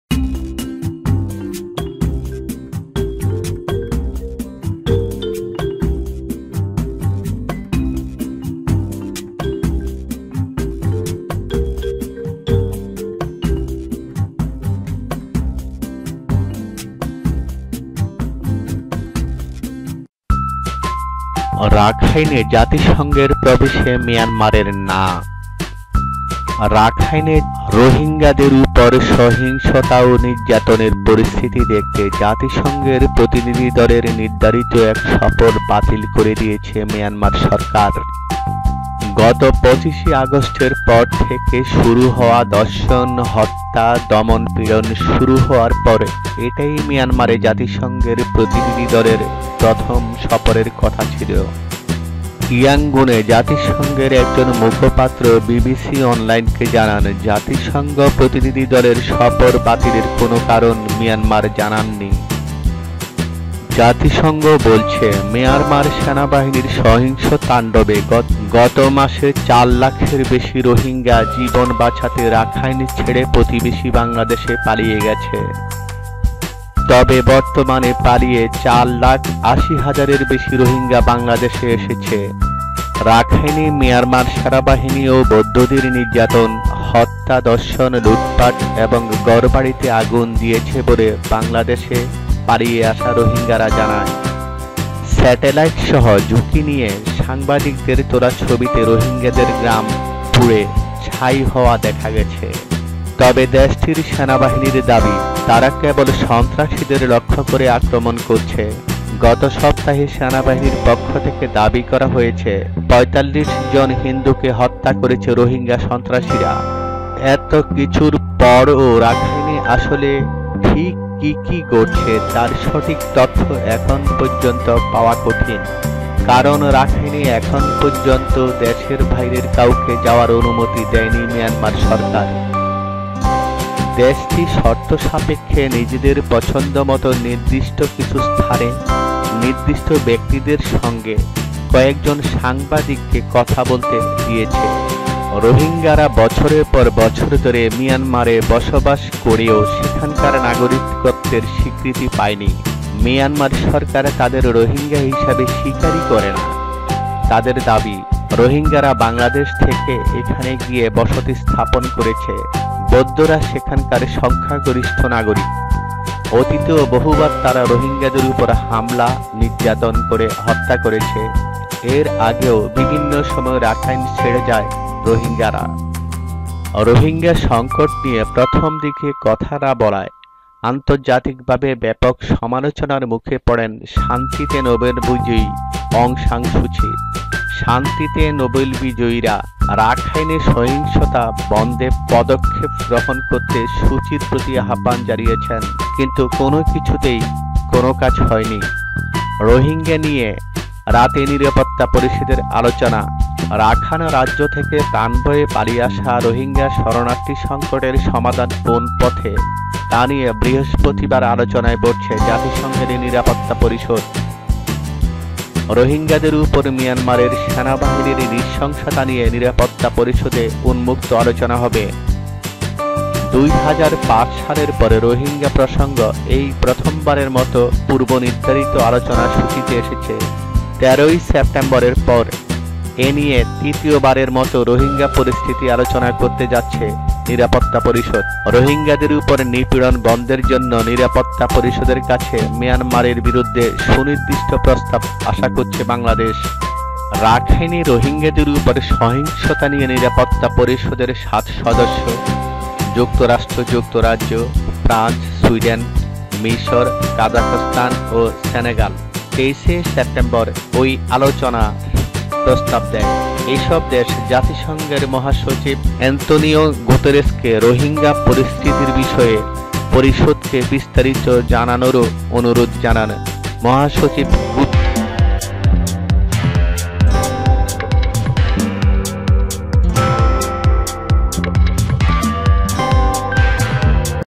Myanmar's 'no' entry to United Nations Rakhine(রাখাইনে জাতিসংঘের প্রবেশে মিয়ানমারের ‘না’)
রাখাইনে জাতিসংঘের প্রবেশে মিয়ানমারের ‘না’
রাখাইনে রোহিঙ্গাদের ওপর সহিংসতা ও নির্যাতনের পরিস্থিতি দেখতে জাতিসংঘের প্রতিনিধিদলের নির্ধারিত এক সফর বাতিল করে দিয়েছে মিয়ানমার সরকার। গত ২৫ আগস্টের পর থেকে শুরু হওয়া ধর্ষণ, হত্যা, দমনপীড়ন শুরু হওয়ার পর এটাই মিয়ানমারে জাতিসংঘের প্রতিনিধিদলের প্রথম সফরের কথা ছিল।
ইয়াঙ্গুনে জাতিসংঘের একজন মুখপাত্র বিবিসি অনলাইনকে জানান, জাতিসংঘ প্রতিনিধিদলের সফর বাতিলের কোনো কারণ মিয়ানমার জানায়নি।
জাতিসংঘ বলছে, মিয়ানমার সেনাবাহিনীর সহিংস তাণ্ডবে গত মাসে চার লাখের বেশি রোহিঙ্গা জীবন বাঁচাতে রাখাইন ছেড়ে প্রতিবেশী বাংলাদেশে পালিয়ে গেছে।
তবে বর্তমানে পালিয়ে ৪ লাখ ৮০ হাজারের বেশি রোহিঙ্গা বাংলাদেশে এসেছে।
রাখাইনে মিয়ানমার সেনাবাহিনী ও বৌদ্ধদের নির্যাতন, হত্যা, ধর্ষণ, লুটপাট এবং ঘরবাড়িতে আগুন দিয়েছে বলে বাংলাদেশ পালিয়ে আসা রোহিঙ্গারা জানায়। স্যাটেলাইটসহ ঝুঁকি নিয়ে সাংবাদিকের তোলা ছবিতে রোহিঙ্গাদের গ্রাম পুড়ে ছাই হওয়া দেখা গেছে।
তবে দেশটির সেনাবাহিনীর দাবি, তারা কেবল সন্ত্রাসীদের লক্ষ্য করে আক্রমণ করছে। গত সপ্তাহে সেনাবাহিনীর পক্ষ থেকে দাবি করা হয়েছে, ৪৫ জন হিন্দুকে হত্যা করেছে রোহিঙ্গা সন্ত্রাসীরা।
এত কিছু পরও রাখাইনে আসলে ঠিক কী কী ঘটছে, তার সঠিক তথ্য এখন পর্যন্ত পাওয়া কঠিন। কারণ, রাখাইনে এখন পর্যন্ত দেশের বাইরের কাউকে যাওয়ার অনুমতি দেয়নি মিয়ানমার সরকার। দেশটি শর্ত সাপেক্ষে নিজেদের পছন্দমতো নির্দিষ্ট কিছু স্থানে নির্দিষ্ট ব্যক্তিদের সঙ্গে কয়েকজন সাংবাদিককে কথা বলতে দিয়েছে।
রোহিঙ্গারা বছরের পর বছর ধরে মিয়ানমারে বসবাস করেও সেখানকার নাগরিকত্বের স্বীকৃতি পায়নি। মিয়ানমার সরকার তাদের রোহিঙ্গা হিসেবে স্বীকারই করে না। তাদের দাবি, রোহিঙ্গারা বাংলাদেশ থেকে সেখানে গিয়ে বসতি স্থাপন করেছে। বৌদ্ধরা সেখানকার সংখ্যাগরিষ্ঠ নাগরিক। অতীতেও বহুবার তারা রোহিঙ্গাদের ওপর হামলা, নির্যাতন করে হত্যা করেছে। এর আগেও বিভিন্ন সময় রাখাইন ছেড়ে যায় রোহিঙ্গারা।
রোহিঙ্গা সংকট নিয়ে প্রথম দিকে কথা না বলায় আন্তর্জাতিকভাবে ব্যাপক সমালোচনায় মুখে পড়েন শান্তিতে নোবেলজয়ী অং সান সু চি। শান্তিতে নোবেলজয়ীরা রাখাইনে সহিংসতা বন্ধে পদক্ষেপ গ্রহণ করতে সু চির প্রতি আহ্বান জানিয়েছেন। কিন্তু কোনো কিছুতেই কোনো কাজ হয়নি।
রোহিঙ্গা নিয়ে রাতে নিরাপত্তা পরিষদের আলোচনা
রাখাইন রাজ্য থেকে প্রাণভয়ে পালিয়ে আসা রোহিঙ্গা শরণার্থী সংকটের সমাধান কোনো পথে, তা নিয়ে বৃহস্পতিবার আলোচনায় বসছে জাতিসংঘের নিরাপত্তা পরিষদ। রোহিঙ্গাদের ওপর মিয়ানমারের সেনাবাহিনীর নৃশংসতা নিয়ে নিরাপত্তা পরিষদে উন্মুক্ত আলোচনা হবে। ২০০৫ সালের পর রোহিঙ্গা প্রসঙ্গ এই প্রথমবারের মতো পূর্বনির্ধারিত আলোচ্যসূচিতে এসেছে। ১৩ সেপ্টেম্বরের পর এ নিয়ে তৃতীয়বারের মতো রোহিঙ্গা পরিস্থিতি আলোচনা করতে যাচ্ছে নিরাপত্তা পরিষদ। রোহিঙ্গাদের ওপর নিপীড়ন বন্ধের জন্য নিরাপত্তা পরিষদের কাছে মিয়ানমারের বিরুদ্ধে সুনির্দিষ্ট প্রস্তাব আশা করছে বাংলাদেশ।
রাখাইনে রোহিঙ্গাদের ওপর সহিংসতা নিয়ে নিরাপত্তা পরিষদের সাত সদস্য—যুক্তরাষ্ট্র, যুক্তরাজ্য, ফ্রান্স, সুইডেন, মিসর, কাজাখস্তান ও সেনেগাল ২৩ সেপ্টেম্বর ওই আলোচনার প্রস্তাব দেয়। এসব দেশ জাতিসংঘের মহাসচিব আন্তোনিও গুতেরেসকে রোহিঙ্গা পরিস্থিতির বিষয়ে পরিষদকে বিস্তারিত জানানোরও অনুরোধ জানায়। মহাসচিব গুতেরেস বৃহস্পতিবার অধিবেশনের শুরুতে এ বিষয়ে বিস্তারিত তুলে ধরবেন। আর অধিবেশন শেষে একটি বিবৃতি প্রচার করা হতে পারে।